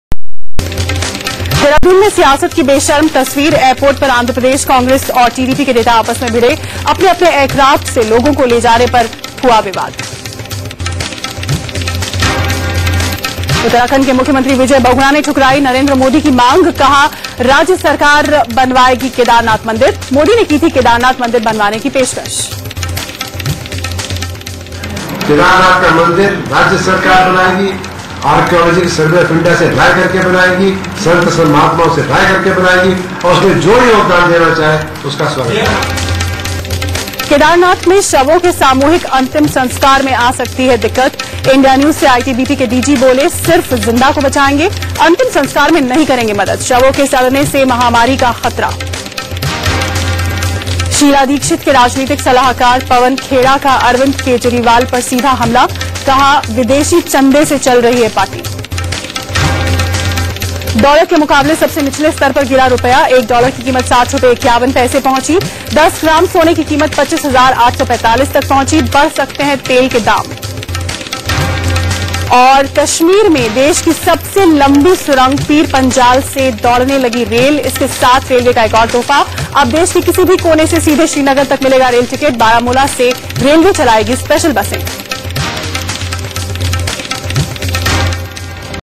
India News: Headlines at 8:00 PM
India News

News 1 : ITBP DG said, " 2 or 3 more days to complete rescue work in Uttarakhand.
News 2 : SP leader said, "Beni babu is an unguided missile"
News 3 : Value of rupee at higest low
News 4 : Prepration of cremation work in Kedarnath